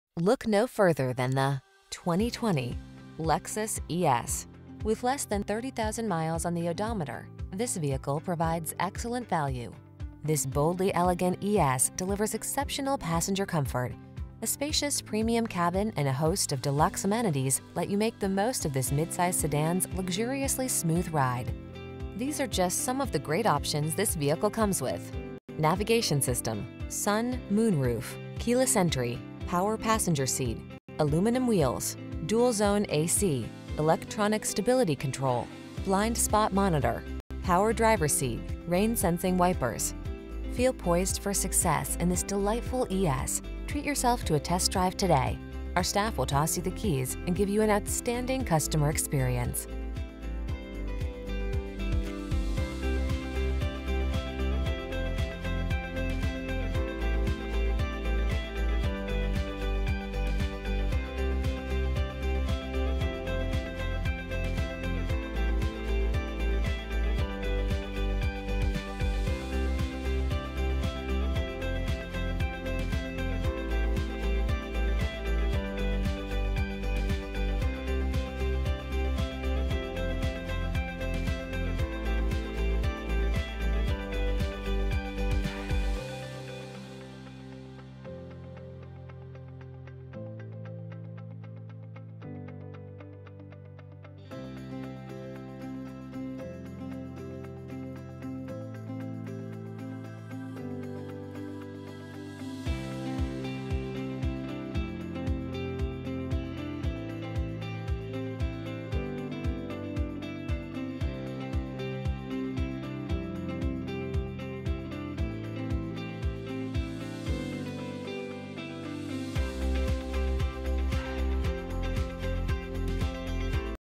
2020 Lexus ES 350-Lexus Of Henderson-Lexus L/Certified
Need an award winning luxury ride with many key features, amenities and certified warranty? Lay your eyes on our L/Certified 2020 Lexus ES 350 ready and waiting to take you for a spin.